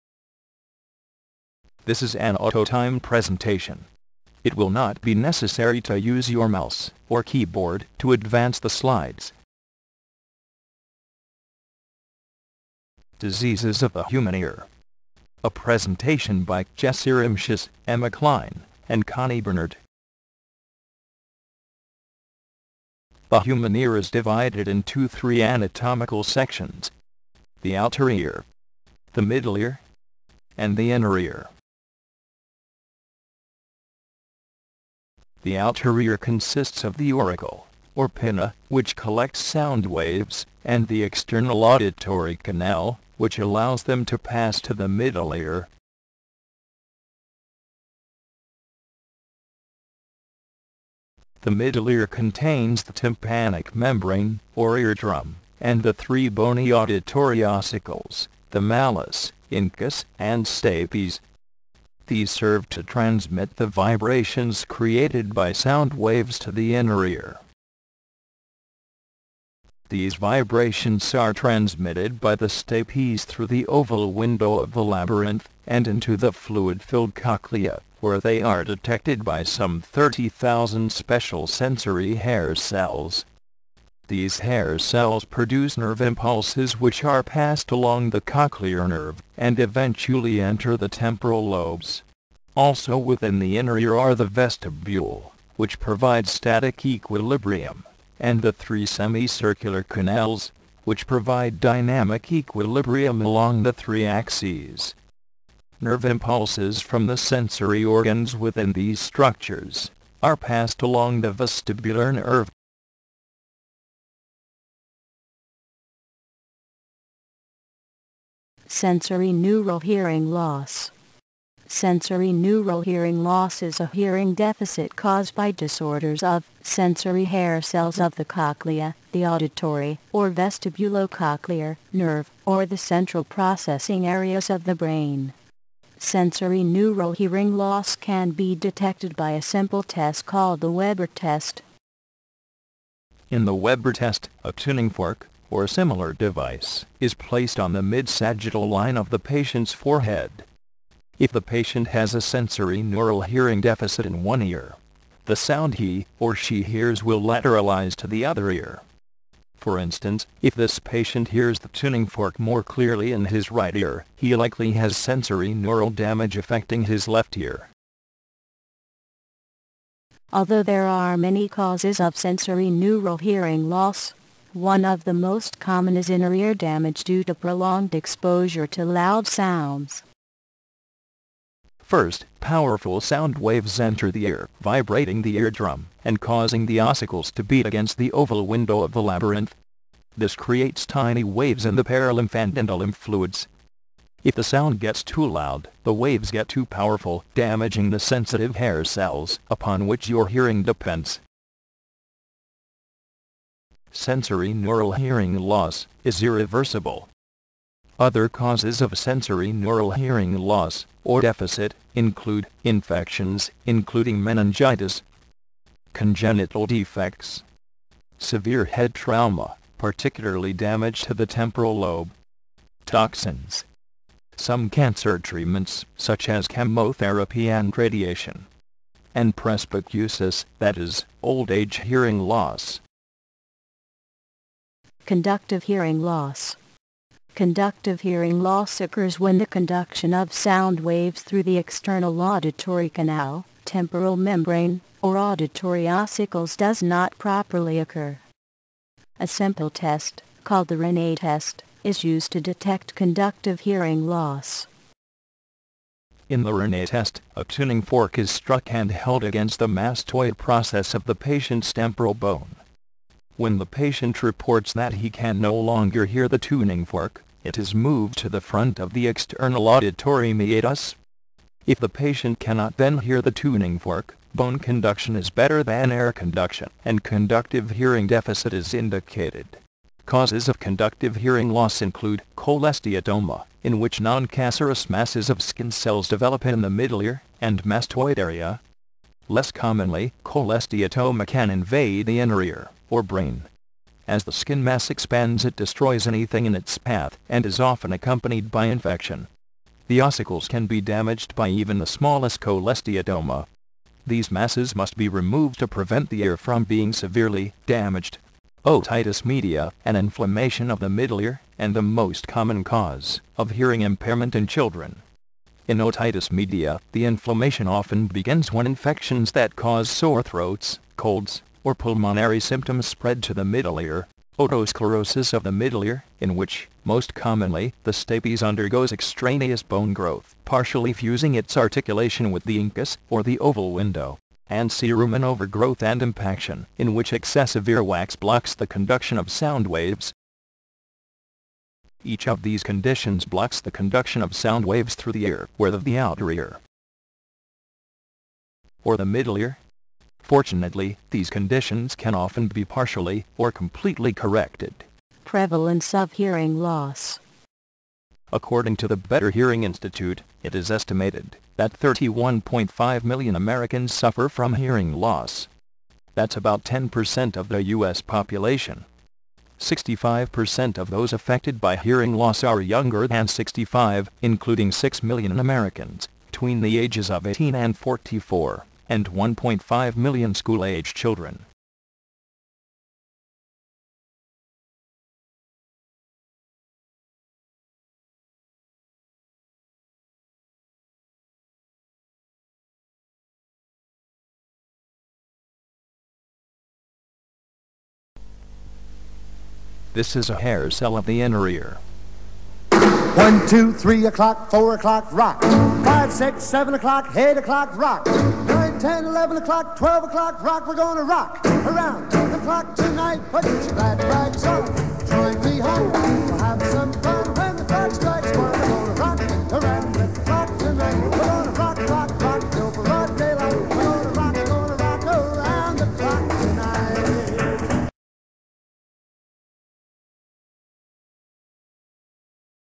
Diseases of the Human Ear.wmv
This is an old e-presentation for an online medical terminology class I had about a year and a half ago.